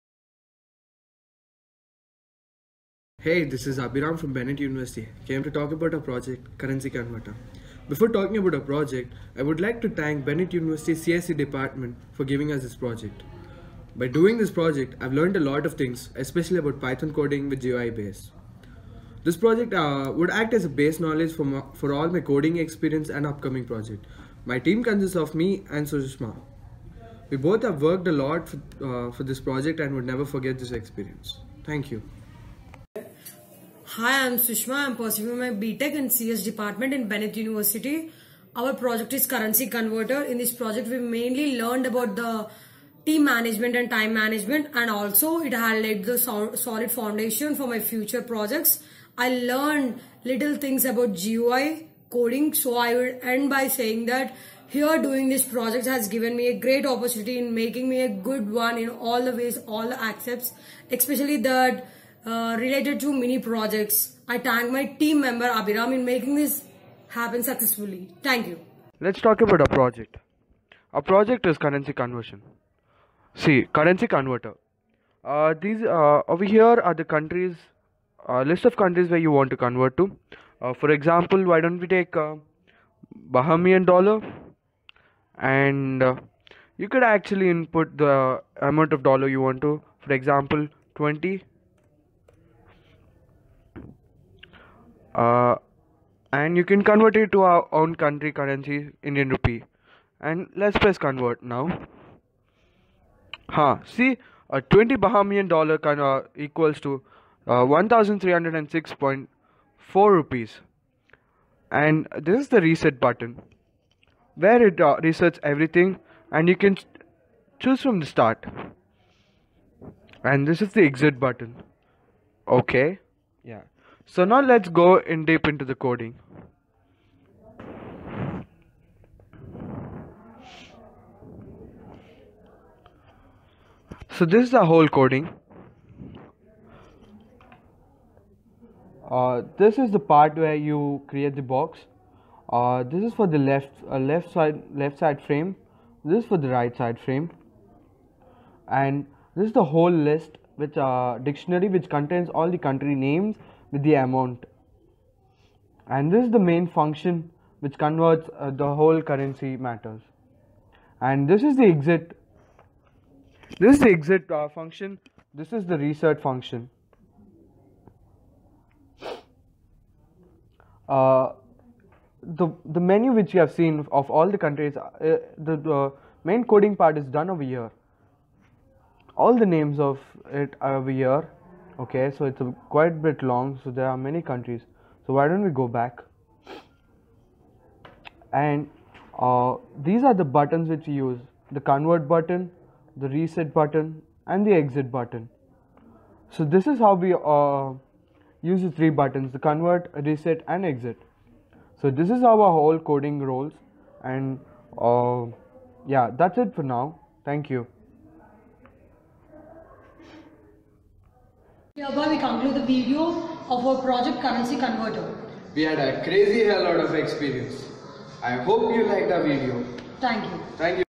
Currency Converter Project by Bennett University Students
Abhiram and Sushma worked a lot and completed their project about currency converter using python 3.5 and above(GUI included)
Please do watch the whole video.